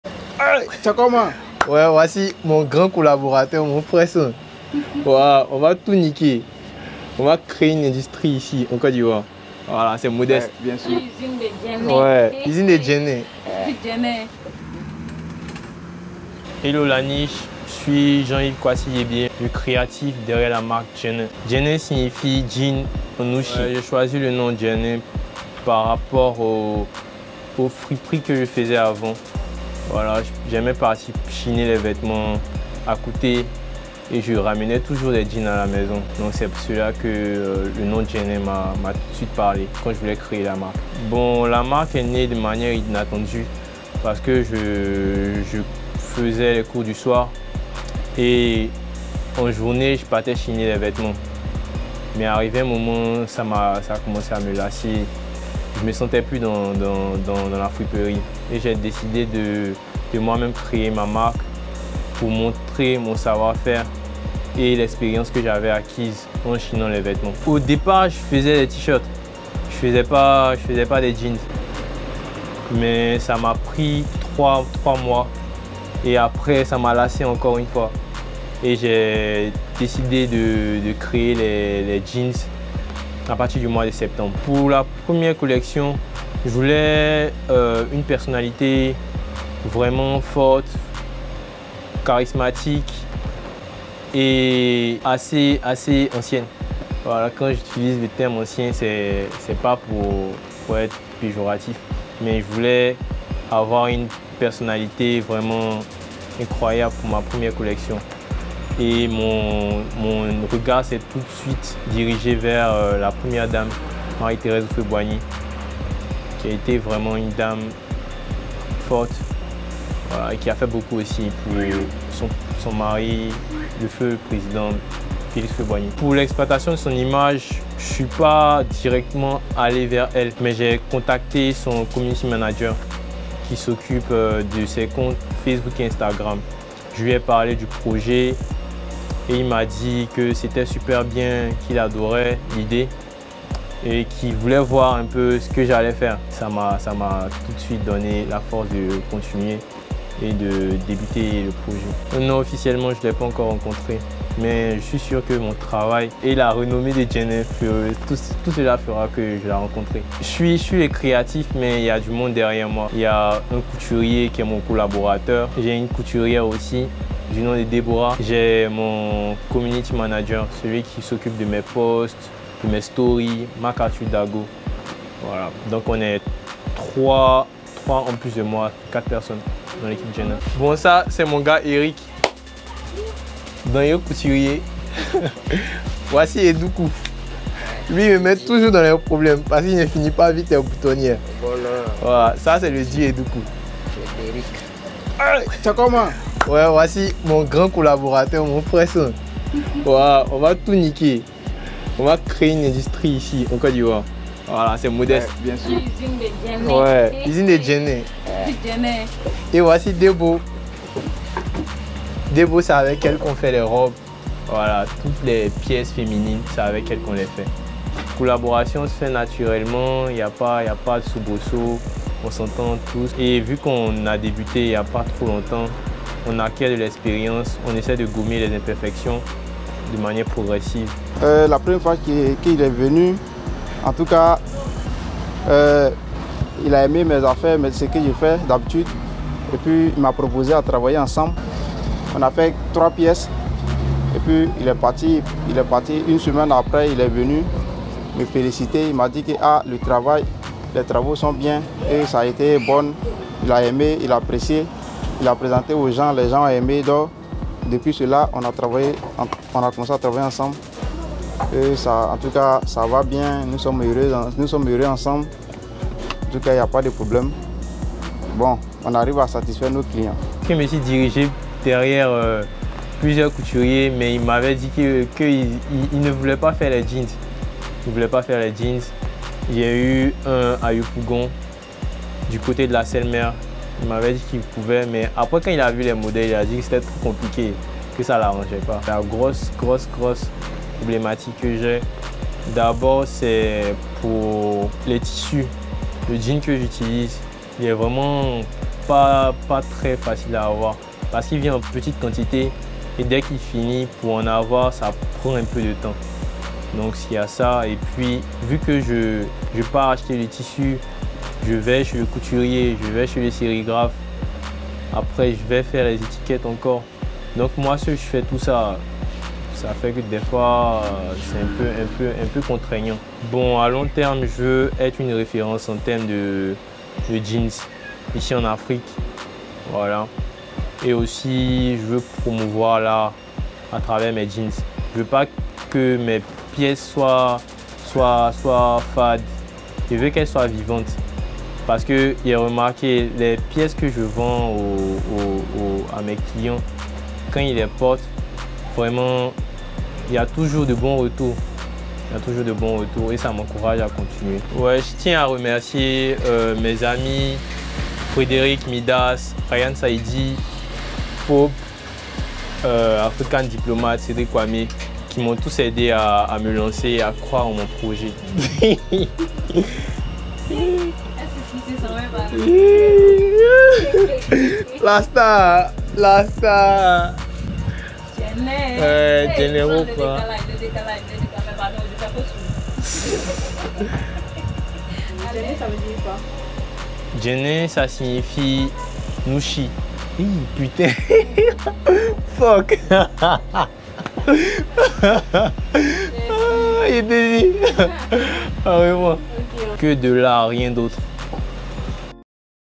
Djainin : "On va créer une industrie, ici en Côte d'Ivoire !"
Rencontre avec Jean-Yves Kouassi, créatif de Yopougon derrière la marque nouvelle marque "Djainin".